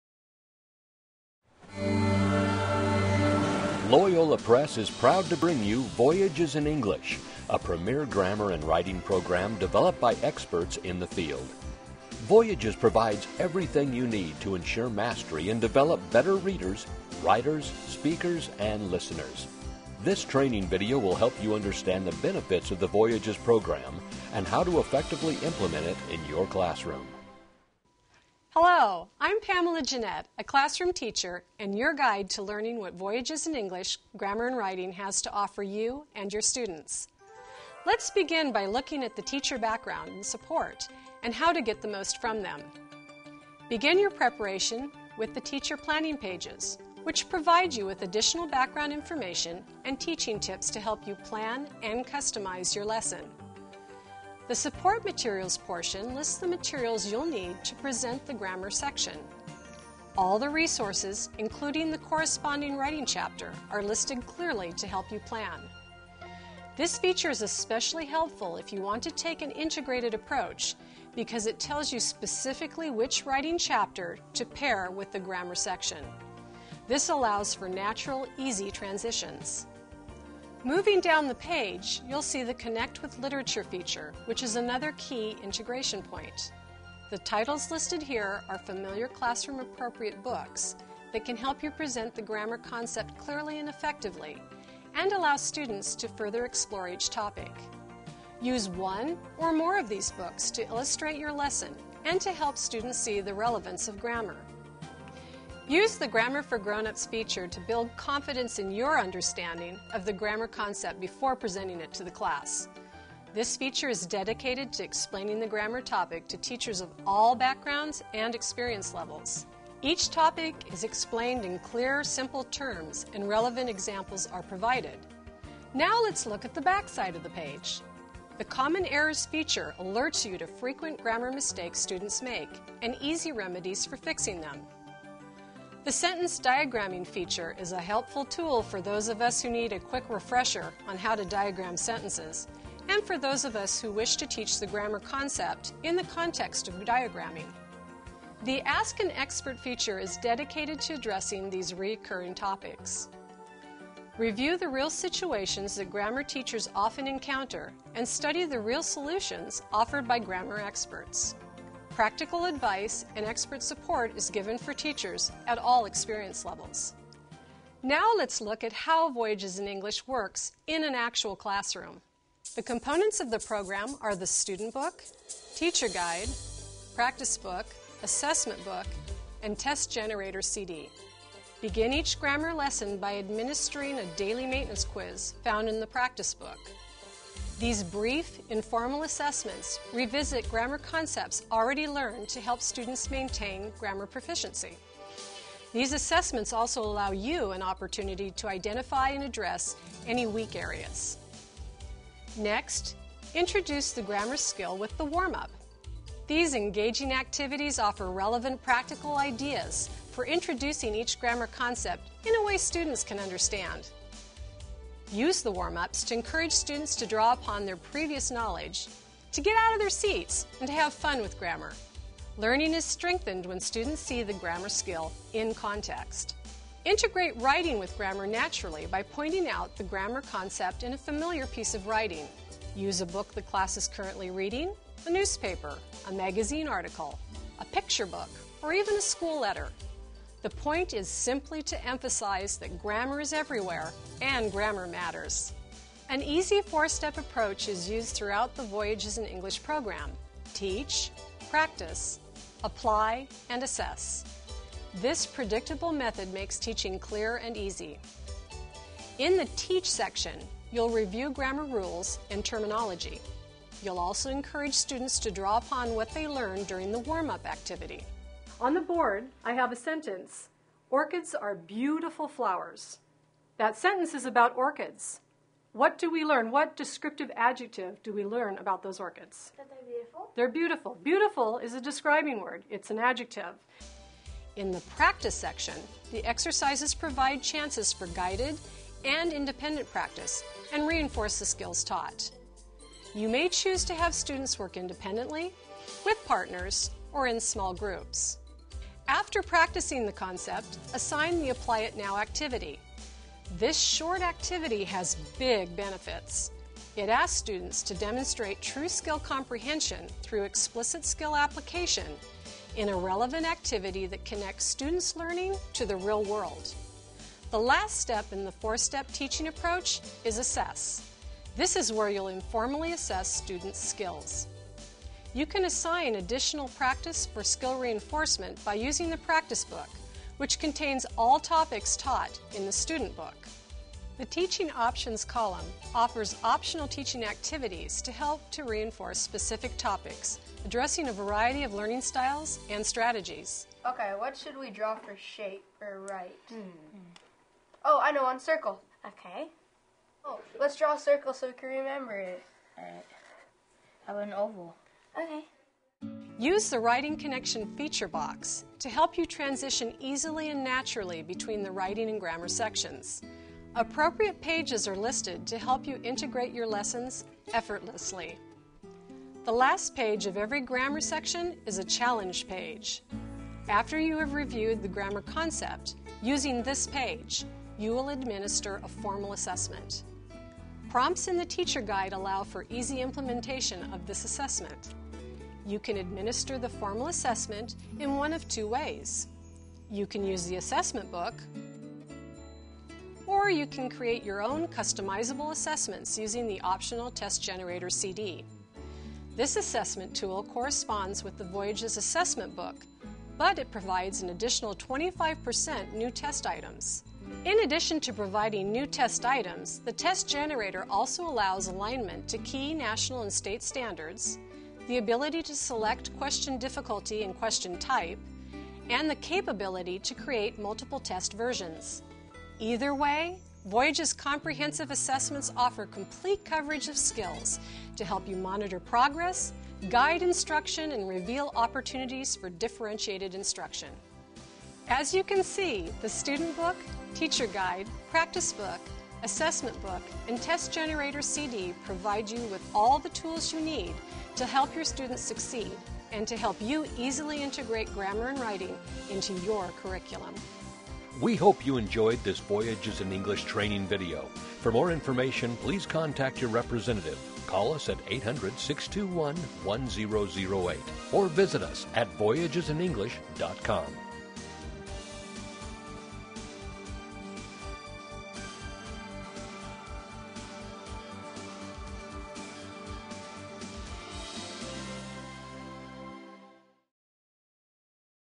Voyages in English - Teaching a grammar lesson
The new 2011 edition of Voyages in English: Grammar and Writing for Grades 3-8 is the result of decades of research and practice by experts in the field of grammar and writing. Responding to the needs of teachers and students, this new edition provides ample opportunities for practice and review to ensure mastery and improved performance on standardized tests.